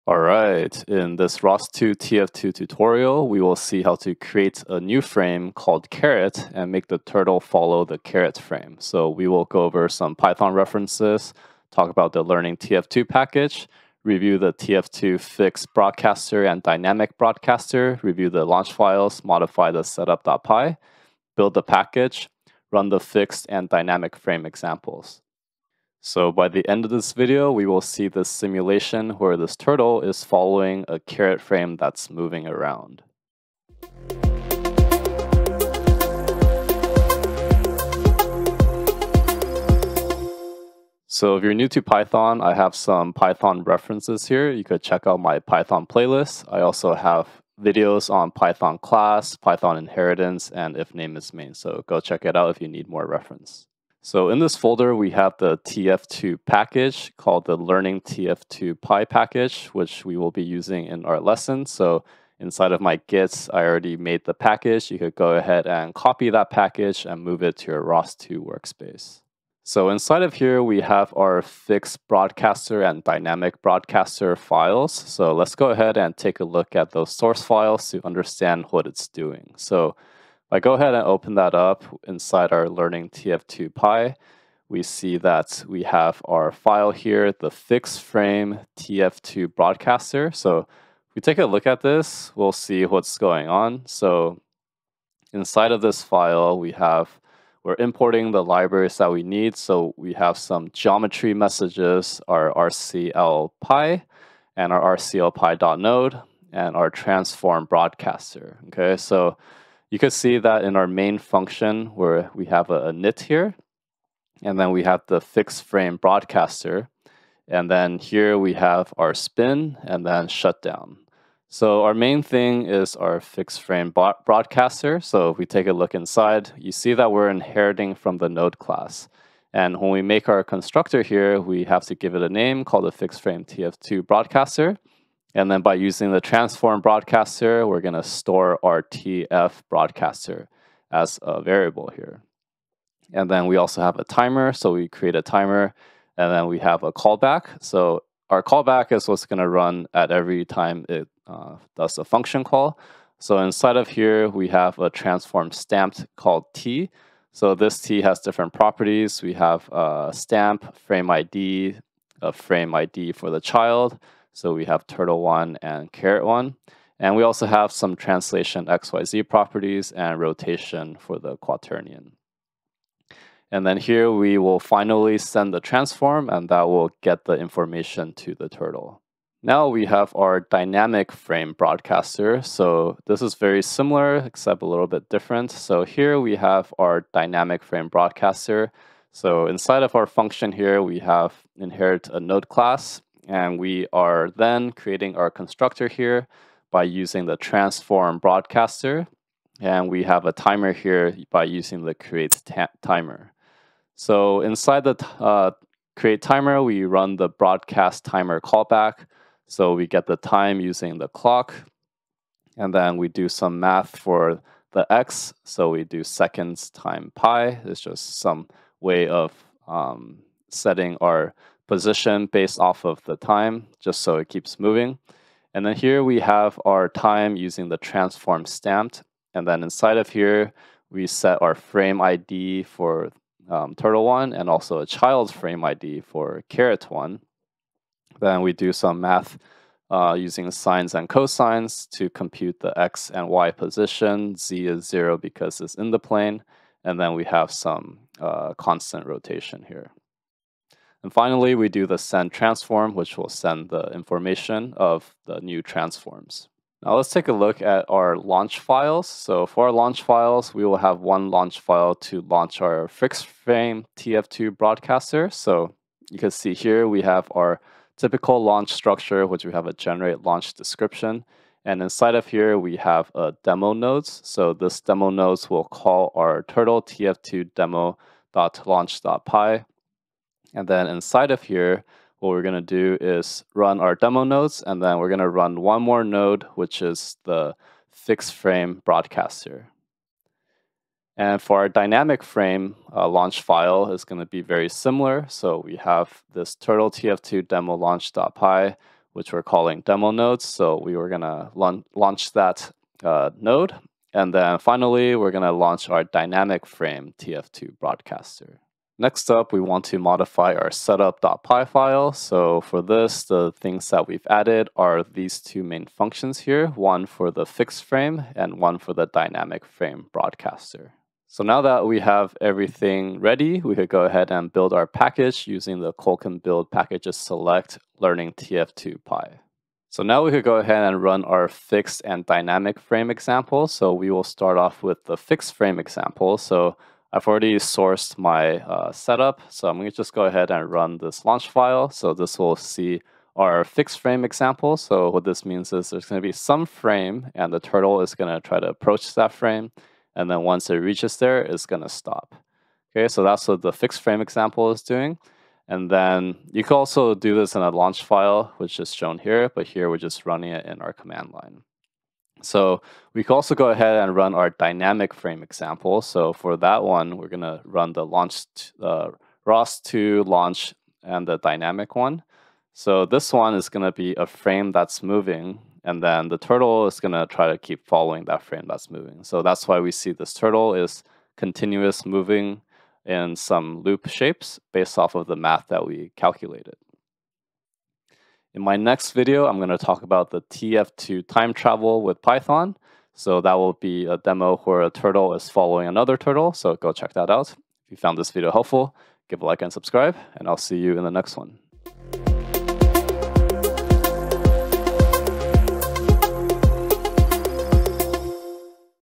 ROS2 tf2 Tutorial - Adding a Frame Python
In this ros2 tf2 tutorial, we will see how to create a new frame and make the turtle follow the frame.

0:00 Introduction
0:40 Python References
0:53 Learning Tf2 Package
1:09 Review tf2 Fixed Broadcaster and Dynamic Broadcaster
4:16 Review Launch Files
5:08 Modify setup.py
5:21 Build package
5:29 Run Fixed and Dynamic Frame Examples